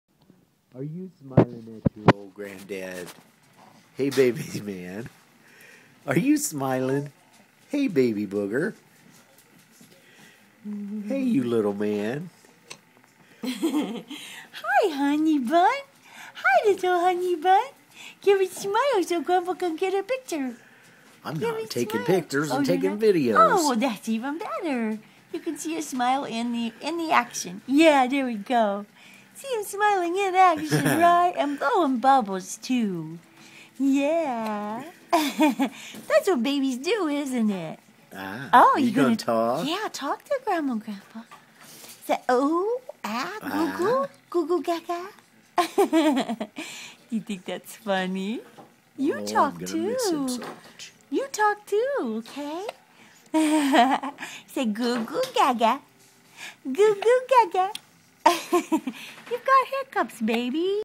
San smiling at grandma
San Adam entertaining grandma & grandpa